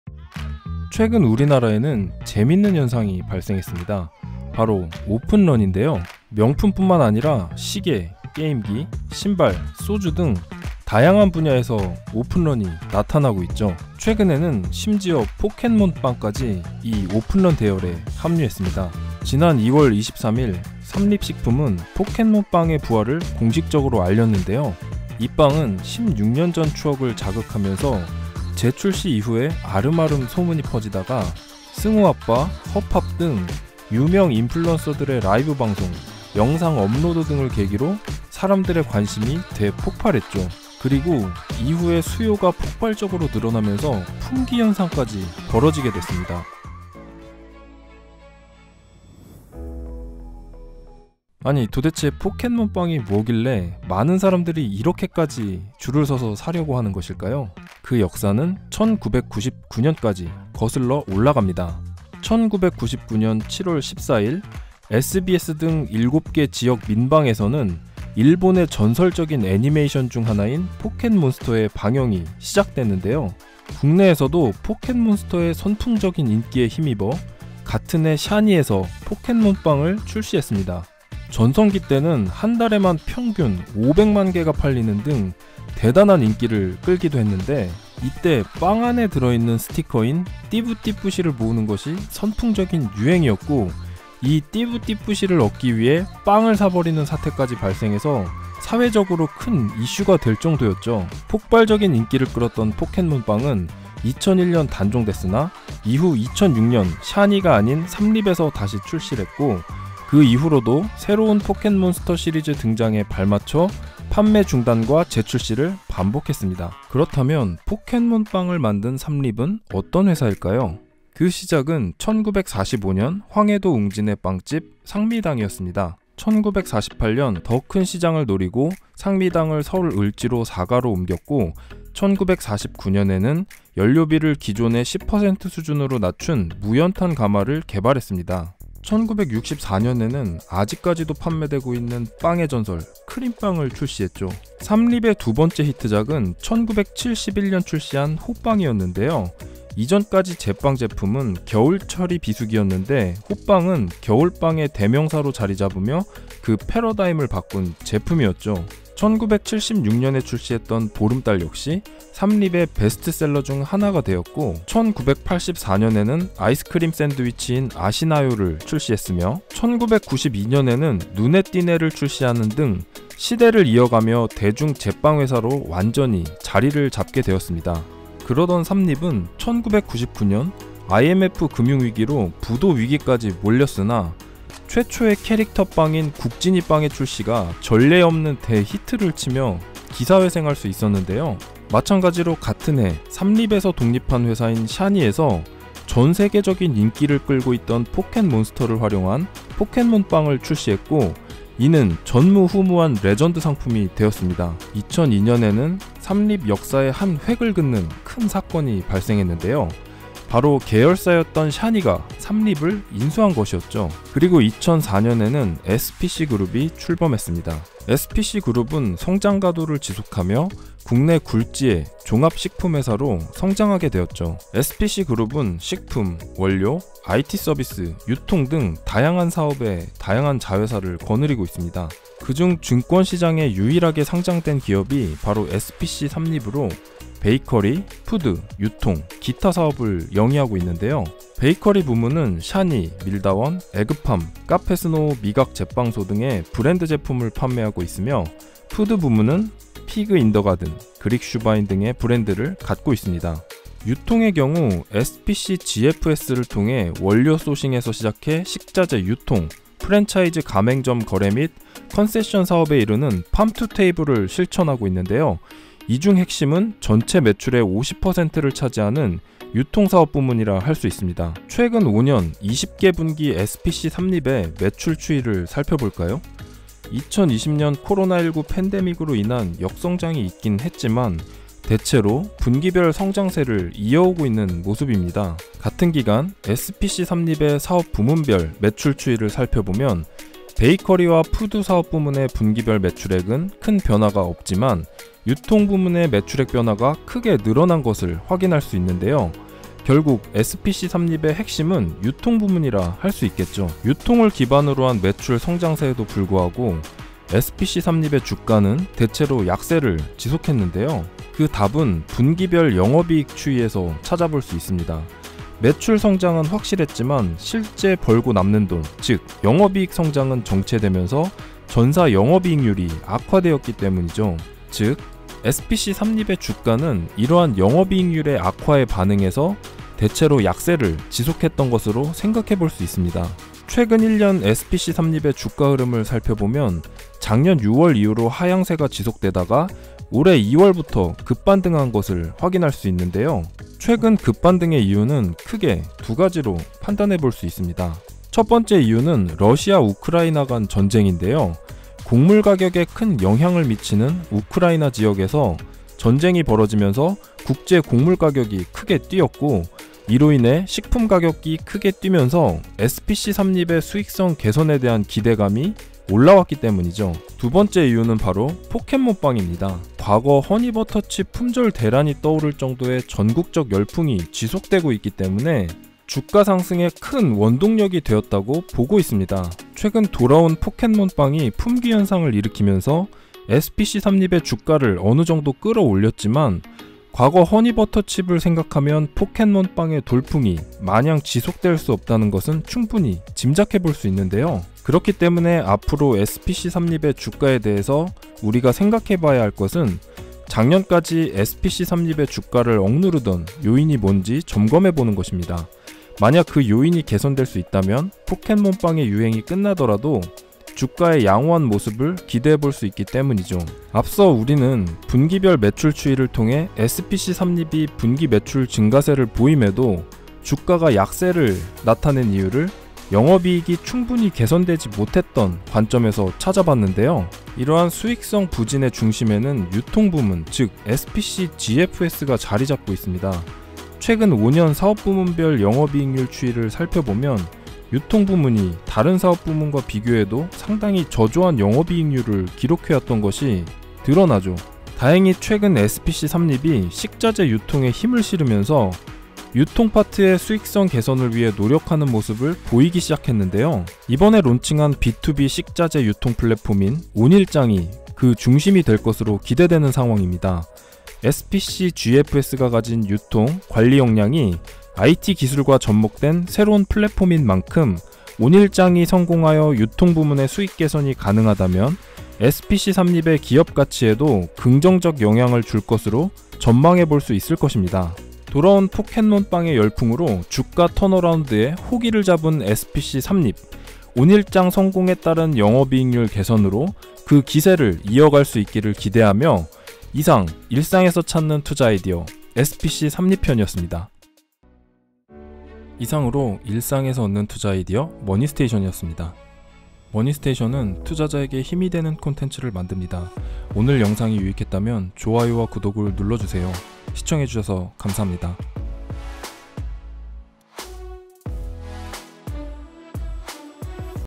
빵 하나가 회사를 살린다 - 포켓몬빵, SPC삼립 편
우린 포켓몬빵을 통해 어떤 투자 아이디어를 얻을 수 있을까요?

돌아온 포켓몬빵을 만드는 SPC삼립은 어떤 회사일까요?
그리고 이번 포켓몬빵의 열풍을 어떻게 투자로 연결할 수 있을까요?